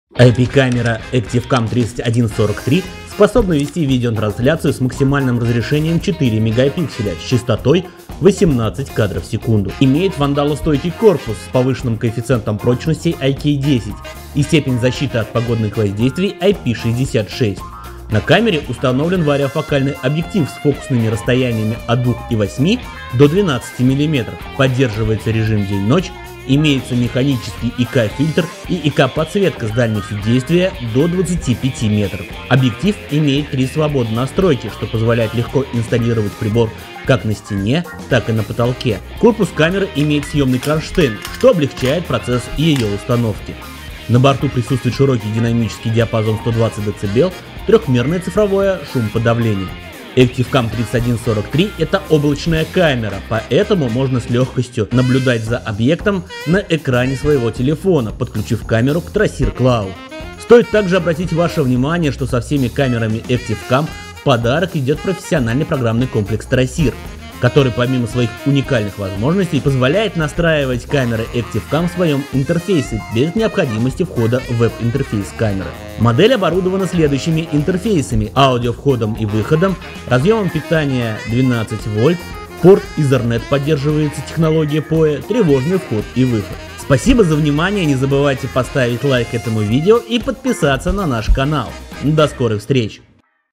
4 Мп купольная камера с вариофокальным объективом ActiveCam AC- D3143VIR2
4 мегапикселя под куполом, реальный wdr в 120 Дц. Облачная камера - поддерживает TRASSIR Cloud и это далеко не все ее достоинства...